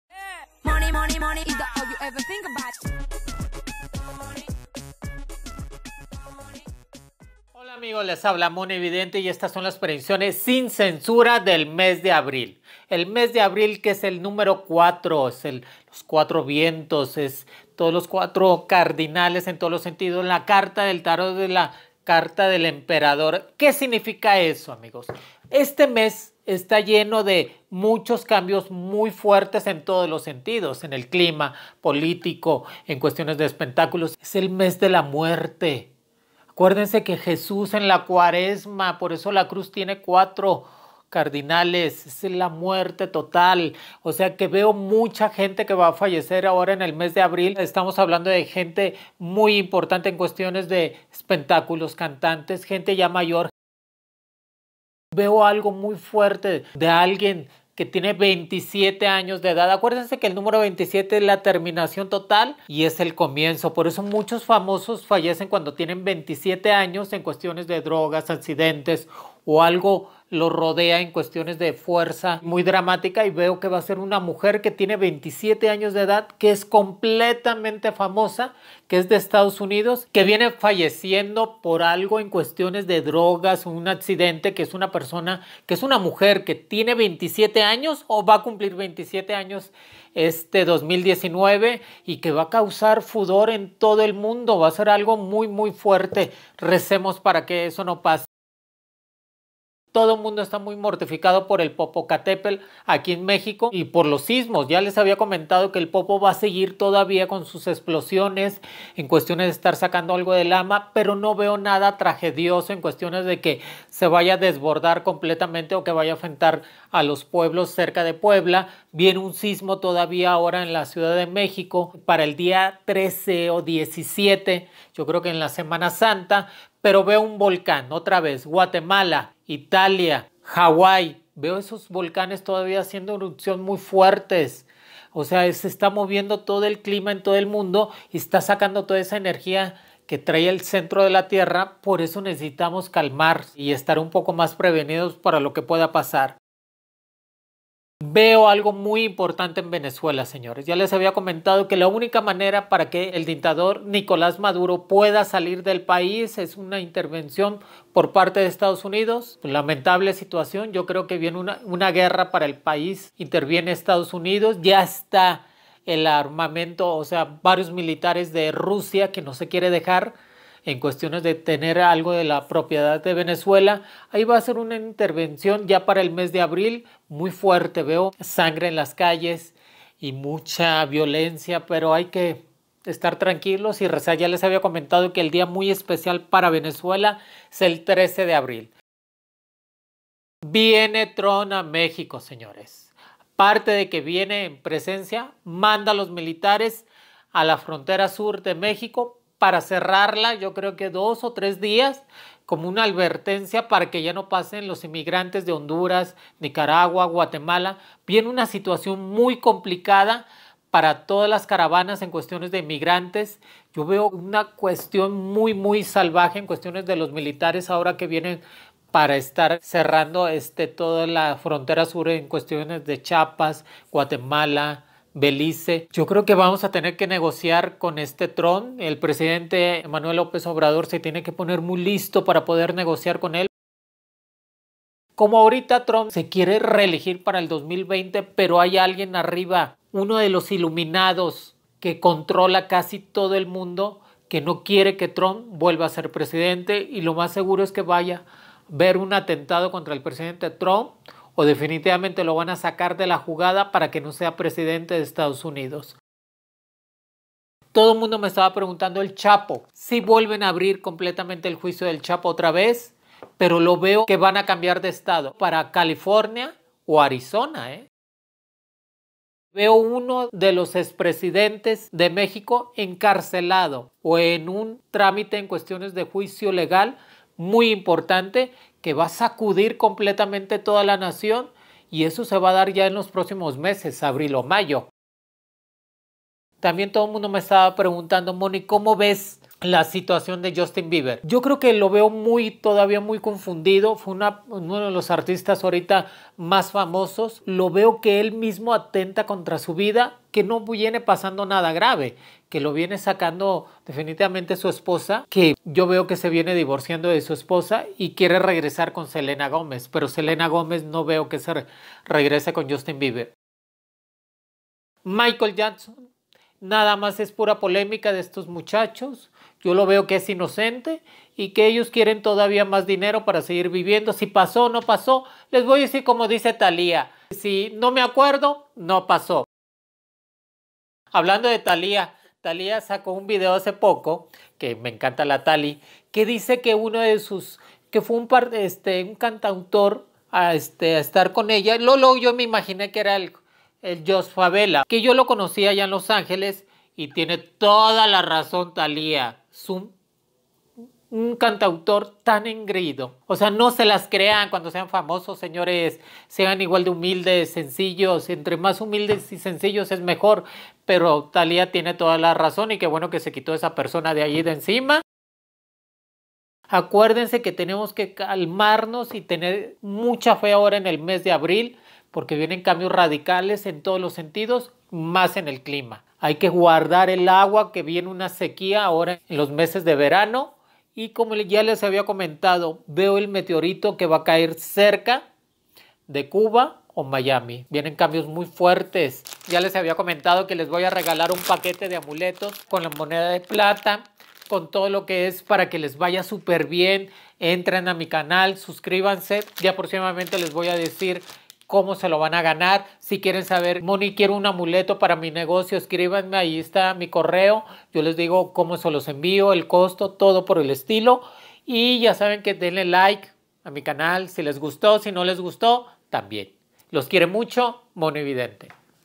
PREDICCIONES DEL MES DE ABRIL
Mhonividente nos comparte sus PREDICCIONES SIN CENSURA para el mes de abril,MES DE LA MUERTE, Popocatépetl, SISMOS, Venezuela, Estados Unidos, Donald Trump, Andres Manuel Lopez Obrador, Chapo Guzman, Encarcelamiento de presidente, Justin Bieber, Michael Jackson, Thalia, Joss Favela, Sequía, Meteorito.